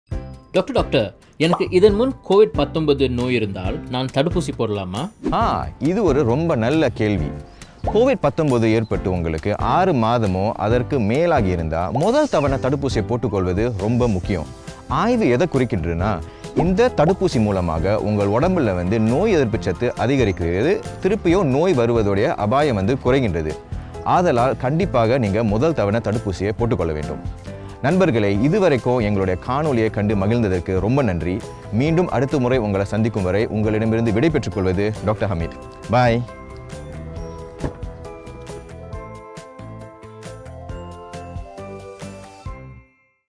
HealthServeTips (Tamil) - Covid-19 FAQ 5/5
"Doctor. I have had a prior Covid-19 infection. Can I be vaccinated?"